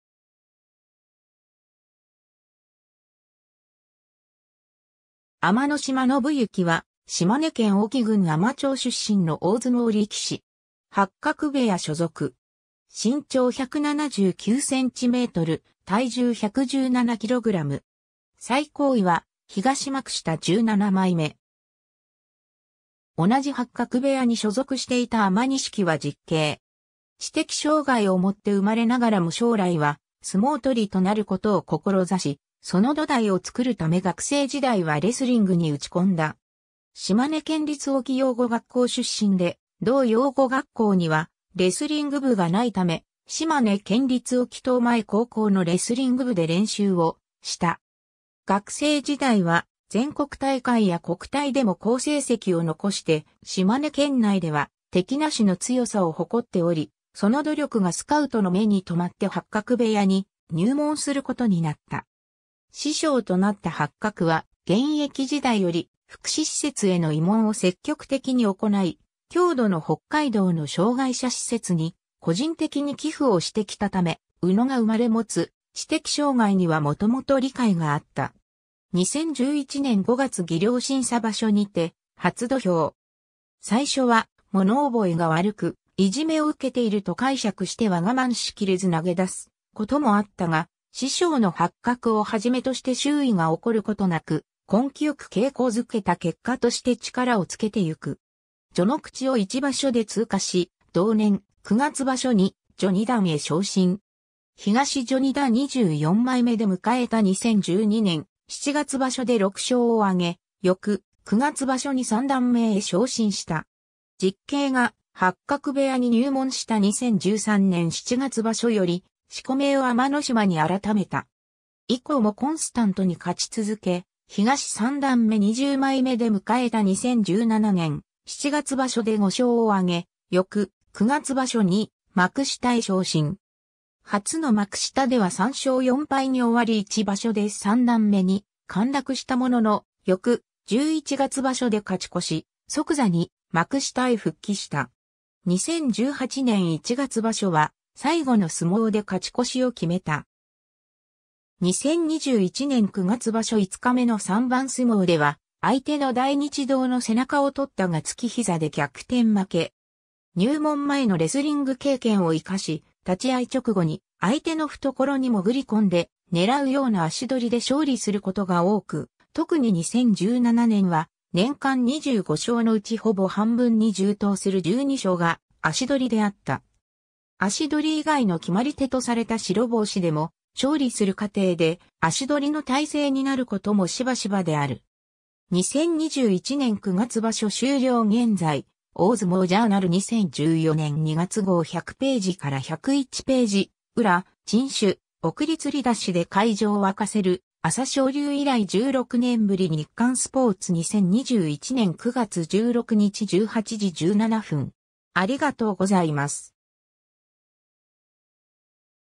海士の島信之
海士の島 信之（あまのしま のぶゆき、1992年10月30日 - ）は、島根県隠岐郡海士町出身の大相撲力士。
八角部屋所属。
最高位は東幕下17枚目（2020年7月場所）。
同じ八角部屋に所属していた海士錦は実兄。
 知的障害を持って生まれながらも将来は相撲取りとなることを志し、その土台を作るため学生時代はレスリングに打ち込んだ。
島根県立隠岐養護学校出身で、同養護学校にはレスリング部が無いため、島根県立隠岐島前高校のレスリング部で練習をした。
学生時代は全国大会や国体でも好成績を残して島根県内では敵なしの強さを誇っており、その努力がスカウトの目に留まって八角部屋に入門することになった。
師匠となった八角は現役時代より福祉施設への慰問を積極的に行い、郷土の北海道の障害者施設に個人的に寄付をしてきたため、宇野が生まれ持つ知的障害には元々理解があった。
 2011年5月技量審査場所にて初土俵。
最初は物覚えが悪く、いじめを受けていると解釈しては我慢しきれず投げ出すこともあったが師匠の八角を初めとして周囲が怒ることなく根気良く稽古付けた結果として力を付けてゆく。
序ノ口を1場所で通過し、同年9月場所に序二段へ昇進。
東序二段24枚目で迎えた2012年7月場所で6勝を挙げ、翌9月場所に三段目へ昇進した。
実兄が八角部屋に入門した2013年7月場所より四股名を海士の島（あまのしま）に改めた。
以降もコンスタントに勝ち続け、東三段目20枚目で迎えた2017年7月場所で5勝を挙げ、翌9月場所に幕下へ昇進。
初の幕下では3勝4敗に終わり1場所で三段目に陥落したものの、翌11月場所で勝ち越し、即座に幕下へ復帰した。
2018年1月場所は最後の相撲で勝ち越しを決めた。
 2021年9月場所5日目の3番相撲では相手の大日堂の背中を取ったがつきひざで逆転負け。
 入門前のレスリング経験を活かし、立ち合い直後に相手の懐に潜り込んで狙うような足取りで勝利することが多く、特に2017年は年間25勝のうちほぼ半分に充当する12勝が足取りであった。
足取り以外の決まり手とされた白星でも、勝利する過程で足取りの体勢になることもしばしばである。
 2021年9月場所終了現在 『大相撲ジャーナル』2014年2月号100頁から101頁 ^ 宇良、珍手「送りつり出し」で会場わかせる　朝青龍以来16年ぶり 日刊スポーツ 2021年9月16日18時17分 (2021年9月18日閲覧)